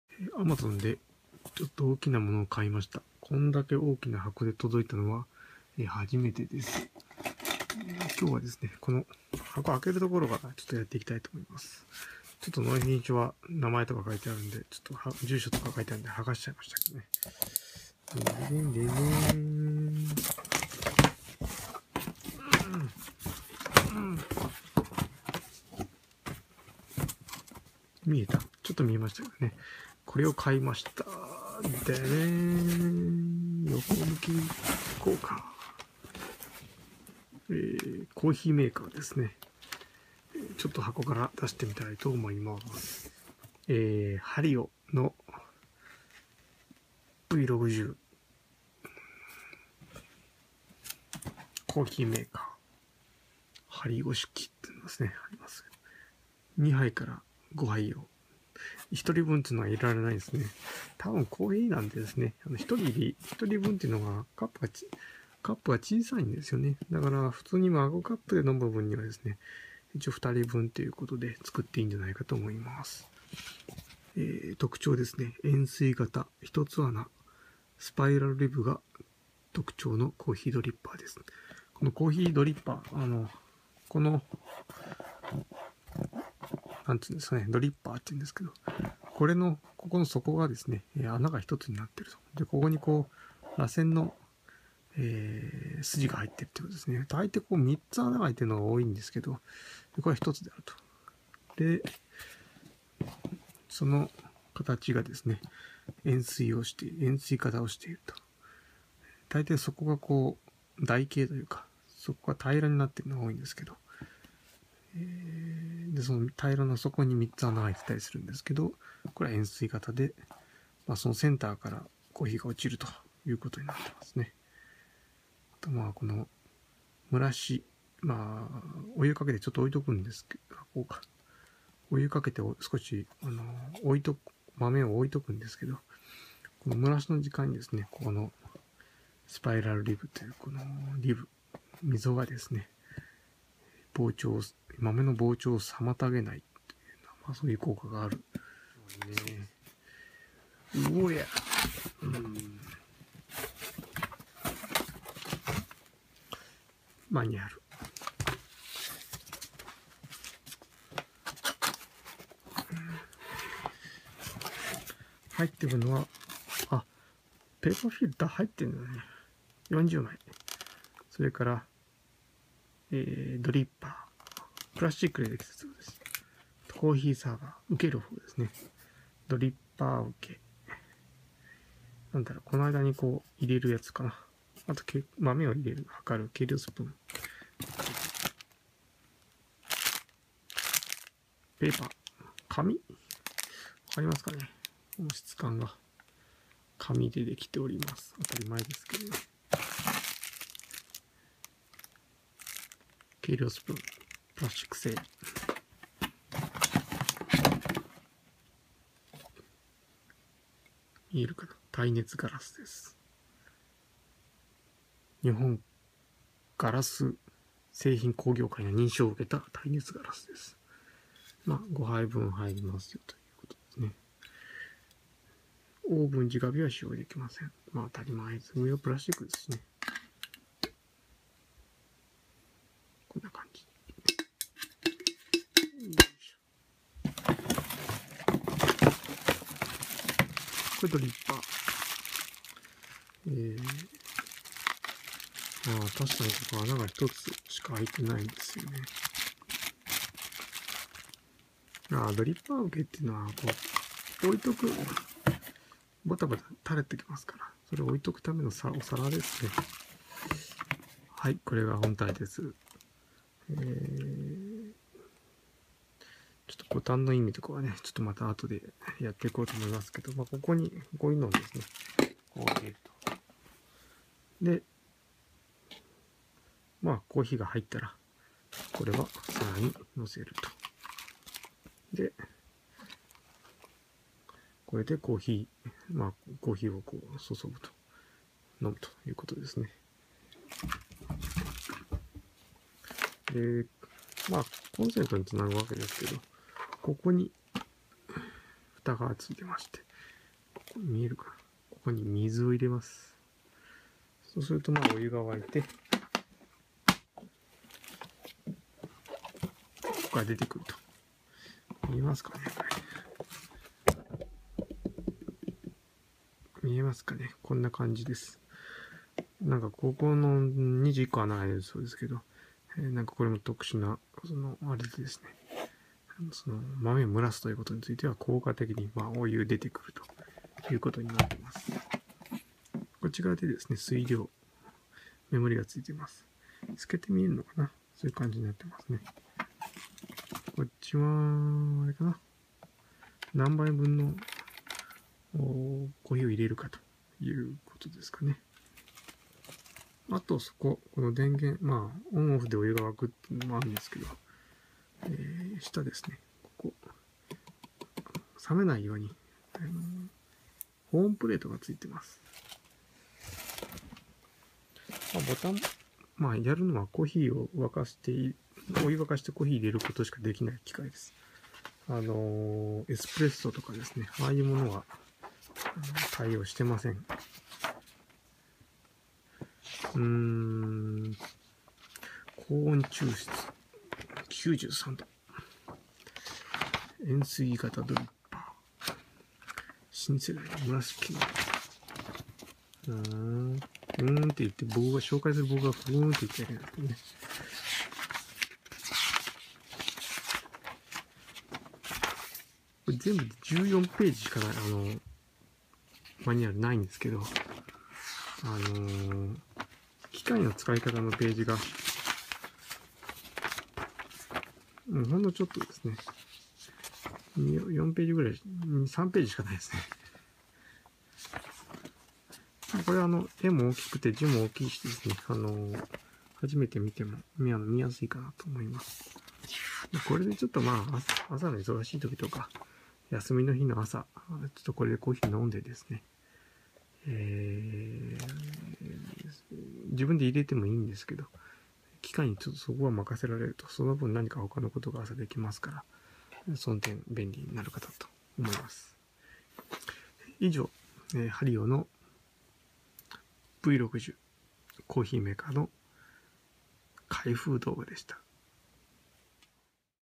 HARIO V60 Coffee Maker（珈琲王）が届いたぞ！！
ハリオの珈琲王を買ったので、それを開封して紹介する動画です。夜中に撮ったので、小声でボソボソ喋ってます。機嫌が悪い訳ではありませんw

iPhone6

【Natsuo1978のメインチャンネル】ぜひ登録してね！